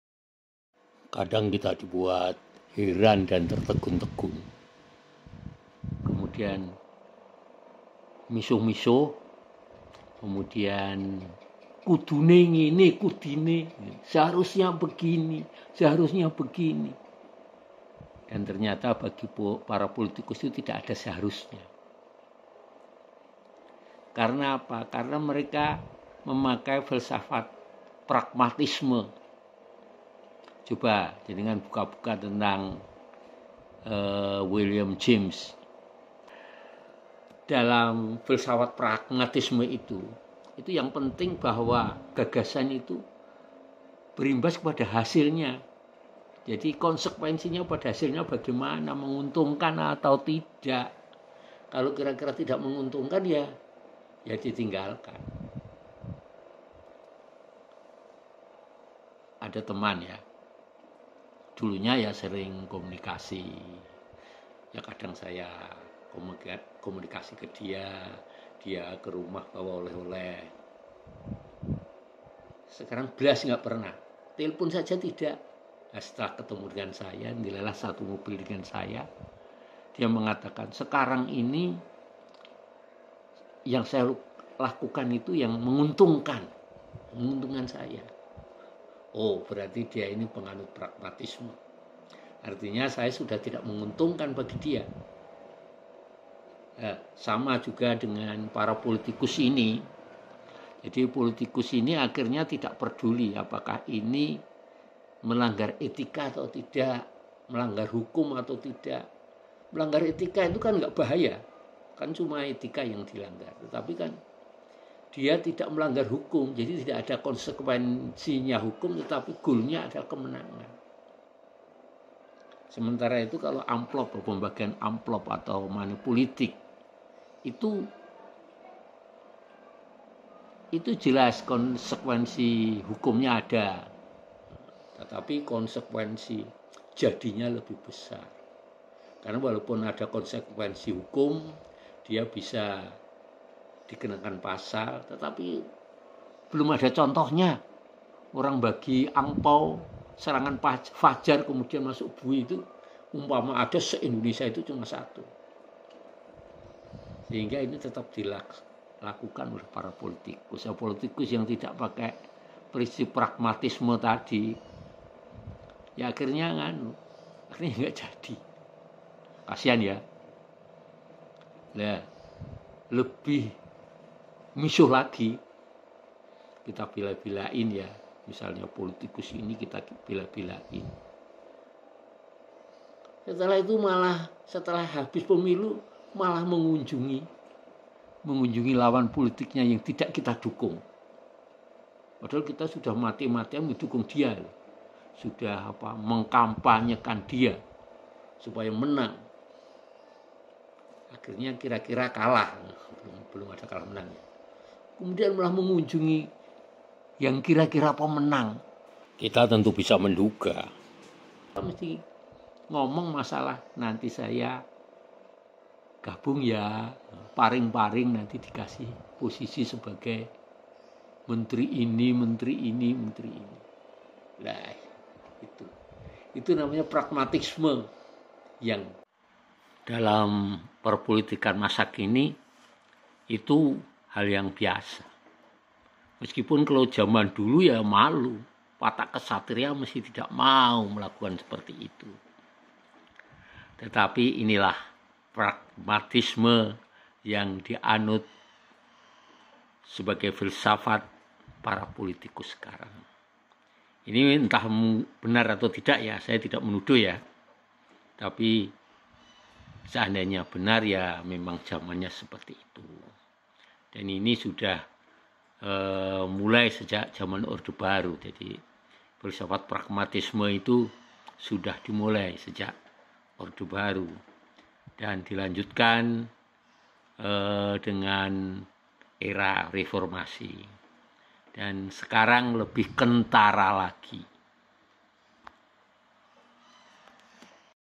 filsafat pragmatisme yang dianut para politikus dewasa ini
pragmatisme adalah suatu pemikiran di mana segala bentuk gagasan dilihat dari manfaatnya. pragmatisme sendiri merupakan cara berpikir praktis di mana mencapai sesuatu dengan cara mudah tidak berbelit-belit.
dewasa ini para politikus lebih memilih aliran pragmatisme ini daripada filsafat etik ataupun estetika maupun  idealisme.
salah satu penggagas pragmatisme ini adalah William James di mana ia berpendapat tentang filsafat ini yang berpaling dari kategori-kategori dari aturan-aturan yang selama ini dianut oleh aliran-aliran filsafat yang lain.